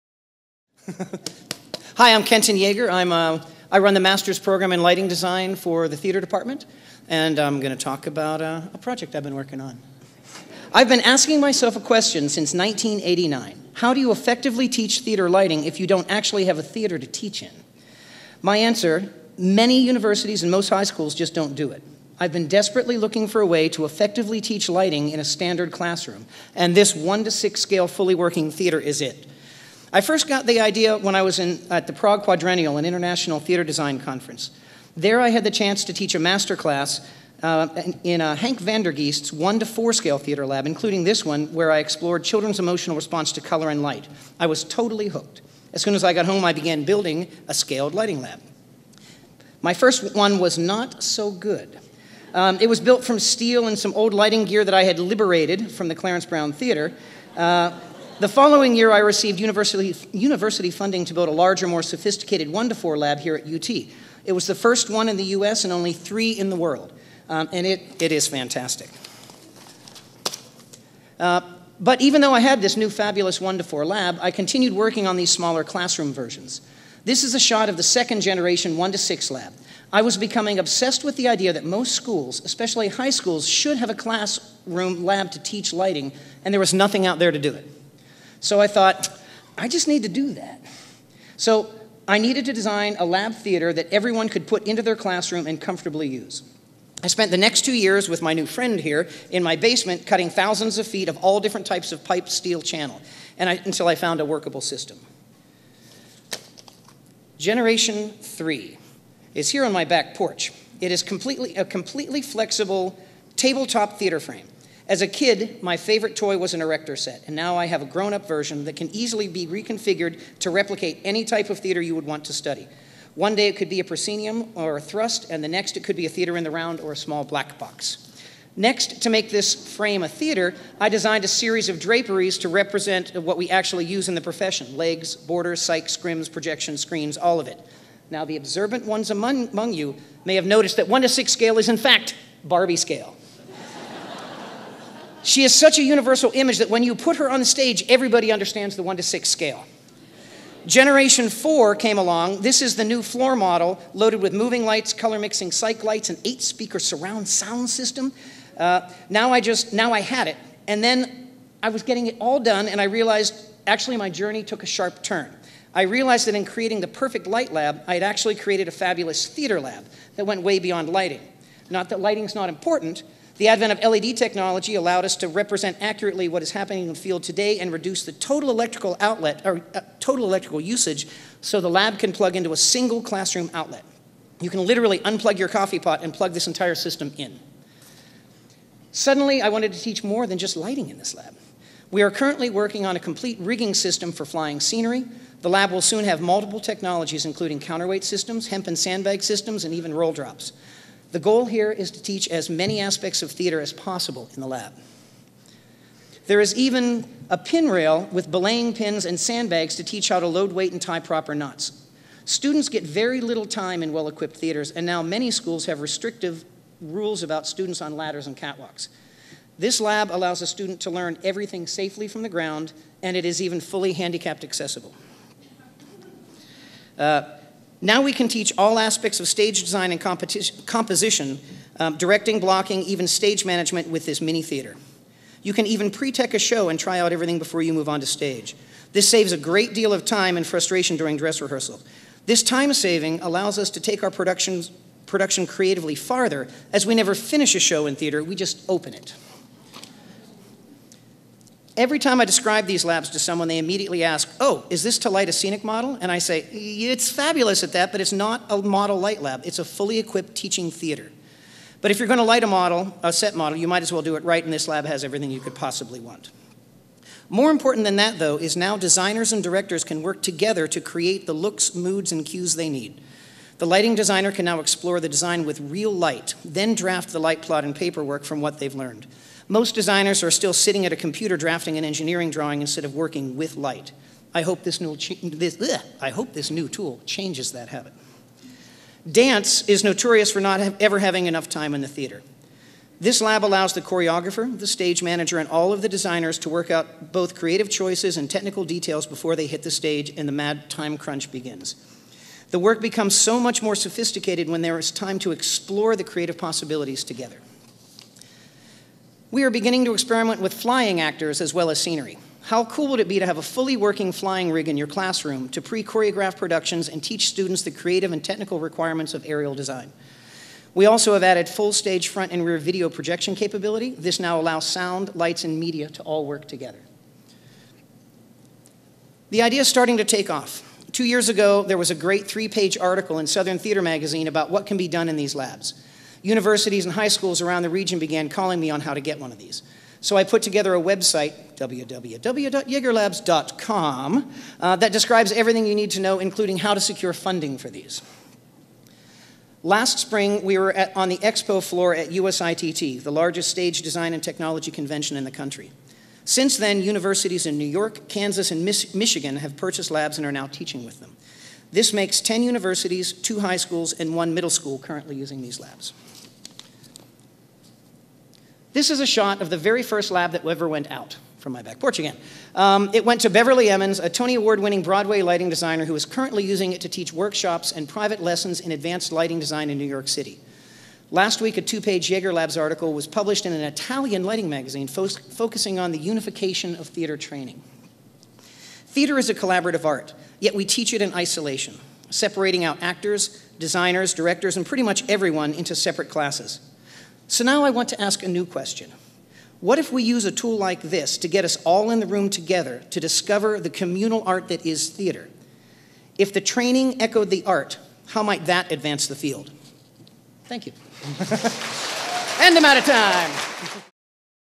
Kenton Yeager: The Development of the Classroom Theatre Lab
In my search to create the perfect theatre lighting lab, I developed an integrated system of teaching theatrical design, directing and stage management. This one-to-six scale theatre allows us to expand beyond lighting and create a complete scaled fully working theatre that fits into a classroom. It has everything an actual theatre has, including lighting, sound, projection, flying and rigging system, legs, borders, drops, scrims, even stage traps and turntables. Called Yeagerlabs, this system has been adopted for use at ten universities, three high schools and a Broadway master class in New York City to teach lighting, scenic, sound, and media design, stage technology, directing and stage management. This system provides a useful educational model designed to develop collaborative skills in our next generation of theatre artists.